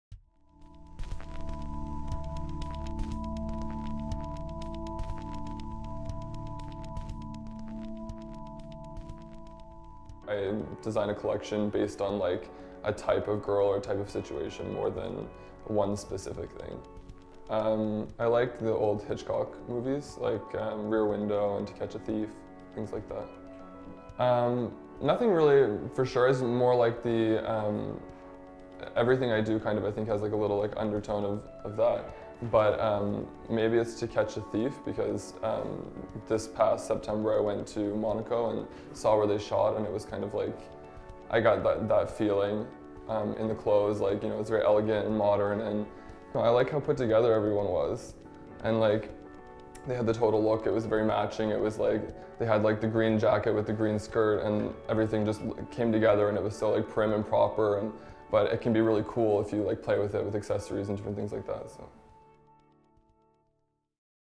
Kobo and THE COLLECTIONS present Matthew Gallagher
Fashion designer Matthew Gallagher talks inspiration and his new collection, which premiered at World MasterCard Fashion Week in Toronto.
Videographer: Adam Beck
Beauty by: Maybelline New York and Redken